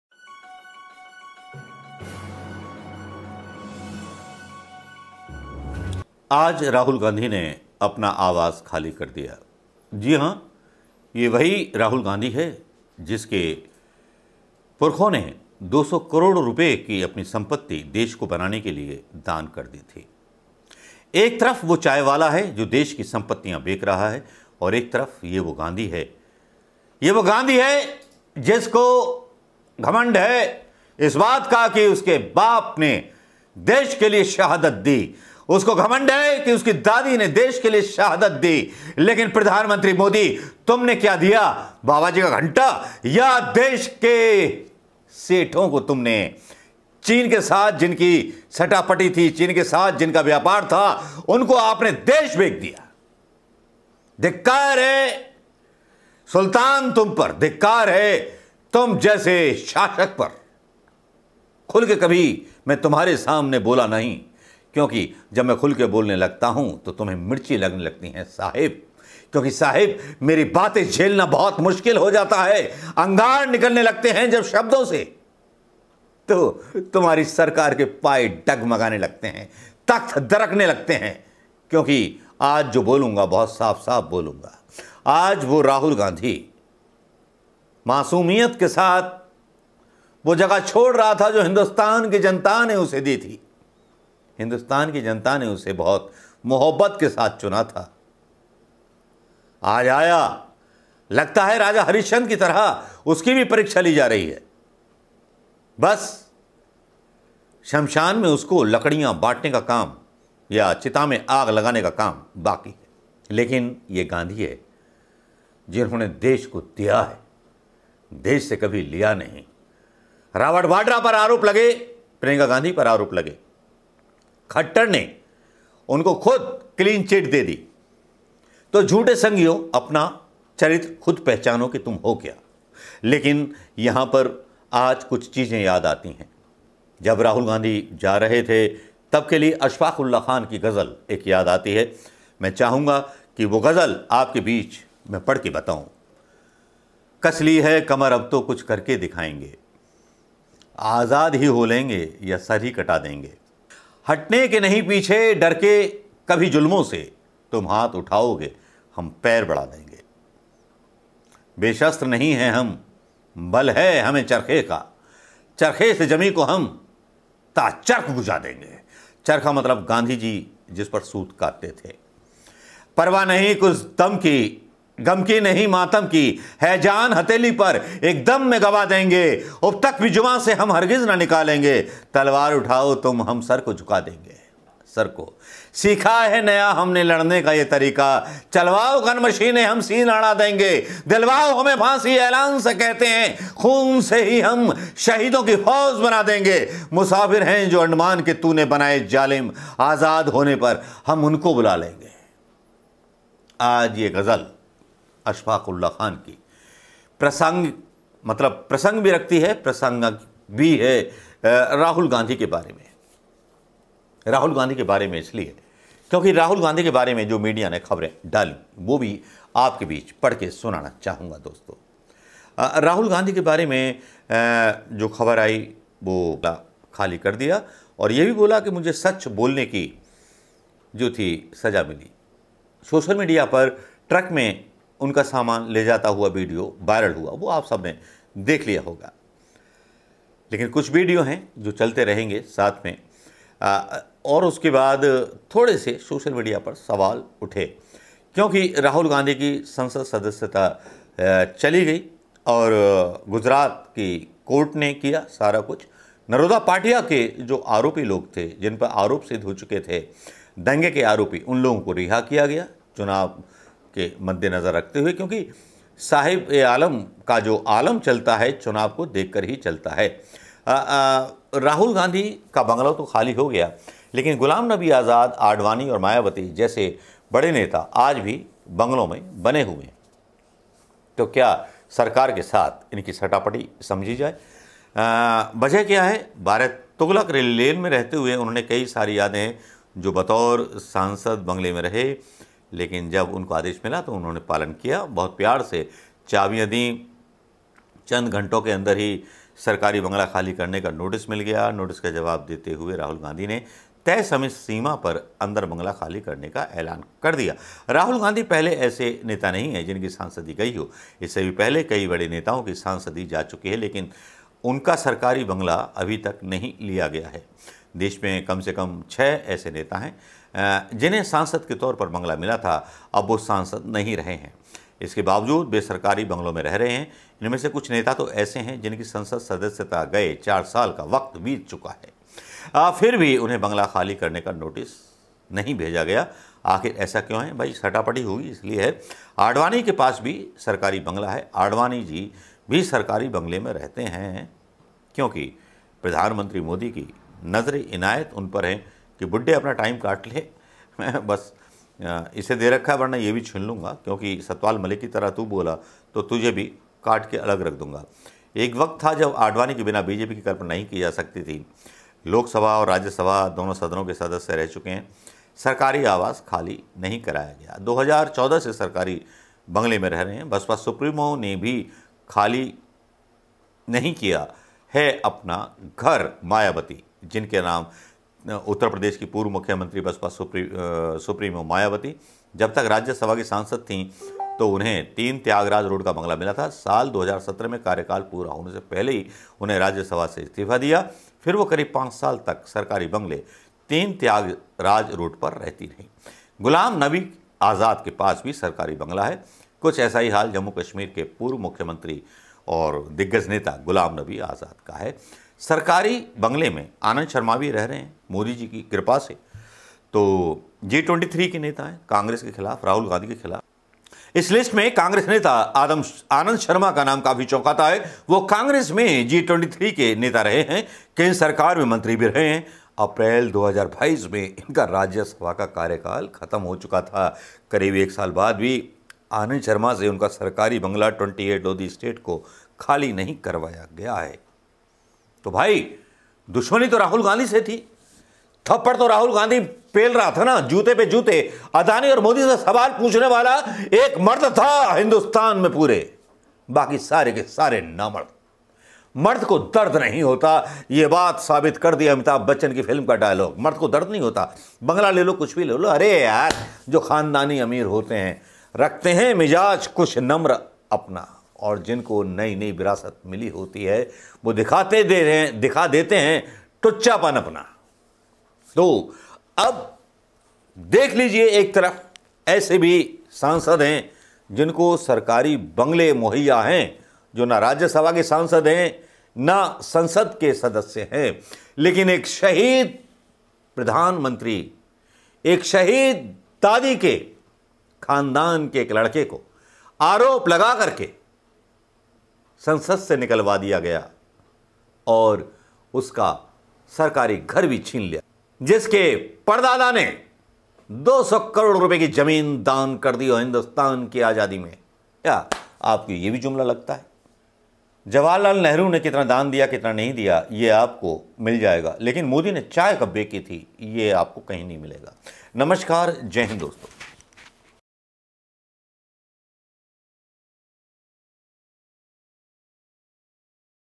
Rahul Gandhi surender Govt House to modi govt ,किसी का भाई किसी की जान राहुल गांधी
राहुल गांधी ने किया घर ख़ाली,राहुल गांधी समाचार